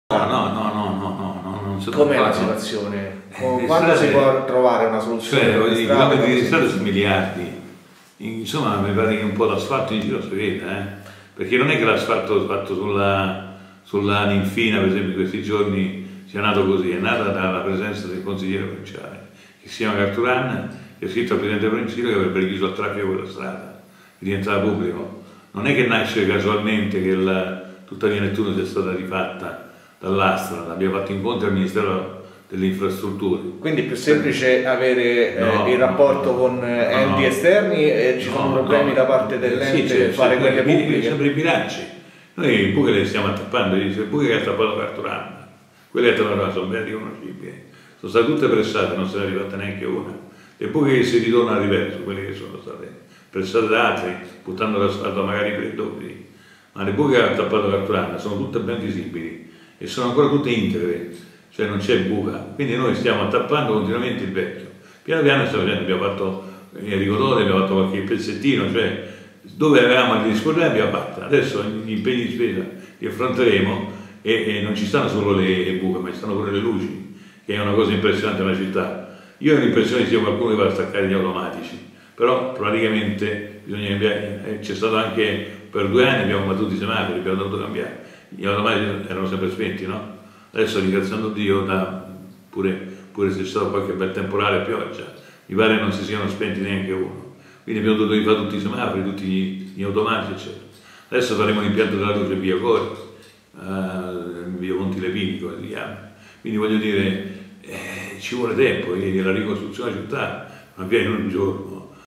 [Intervista] Buche in strada: la versione del sindaco
Cisterna di Latina
Le buche in strada. Lei più di qualche volta ha promesso la risoluzione del problema. Ci vuole dare un’altra tempistica? La risposta di Mauro Carturan